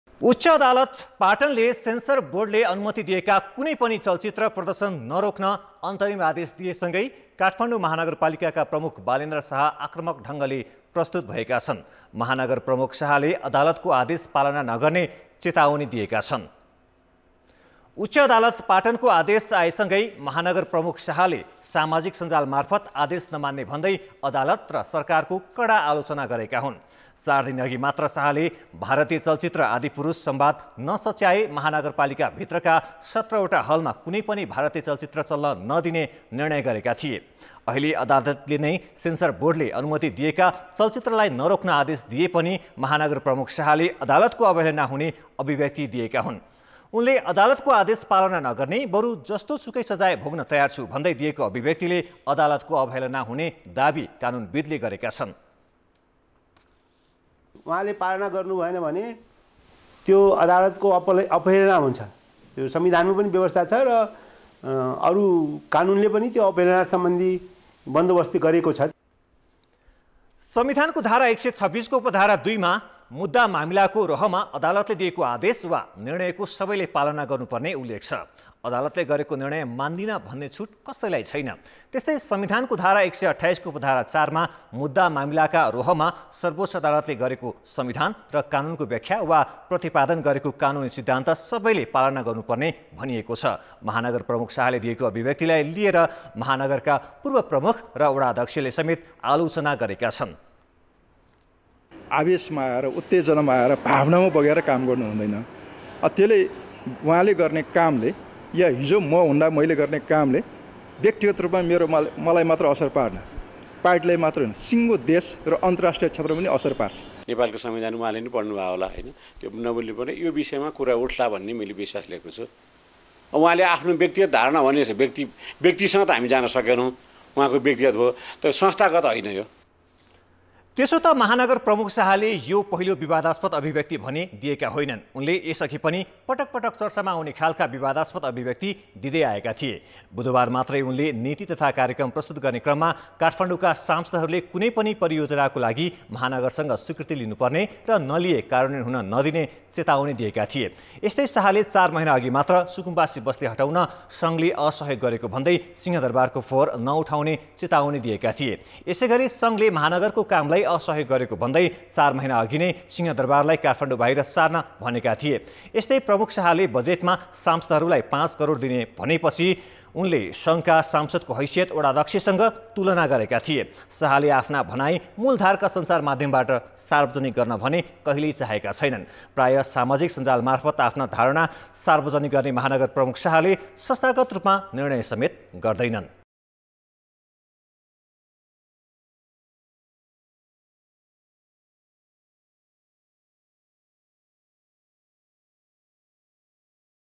चलचित्रको प्रदर्शन नरोक्न उच्च अदालतकाे आदेशप्रति मेयर बालेन्द्र आक्रामक | Kantipur Samachar
उच्च अदालत पाटनले सेन्सर बोर्डले अनुमति दिएका कुनै पनि चलचित्र प्रदर्शन नरोक्न अन्तरिम आदेश दिएसँगै काठमाडौं महानगरपालिकाका प्रमुख बालेन्द्र शाह आक्रामक ढंगले प्रस्तुत् भएका छन् । महानगर प्रमुख शाहले अदालतको आदेश पालना नगर्ने चेतावनी दिएका छन् ।